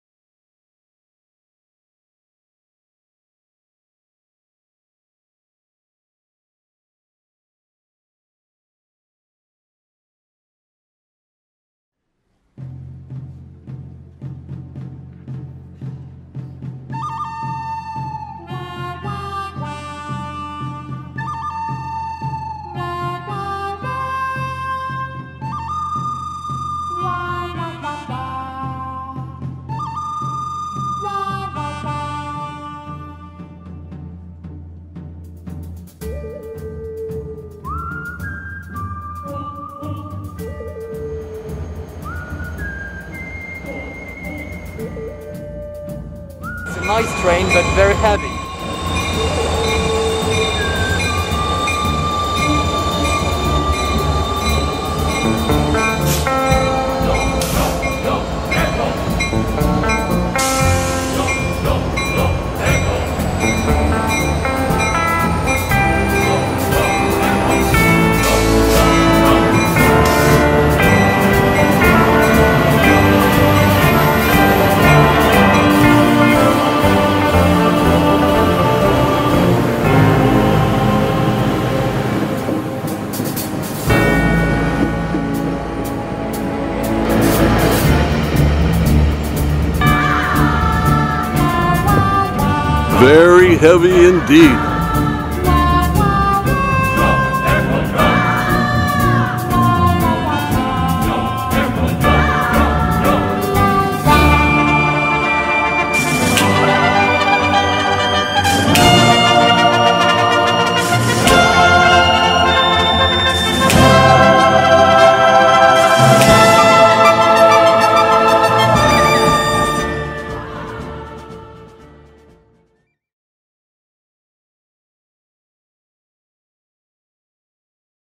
MIKE AT THE MIC = A VERY HEAVY TRAIN featuring The Unknown Masked Man
Mike records another night time train which is - according to the Unknown Masked Man - "A very heavy train."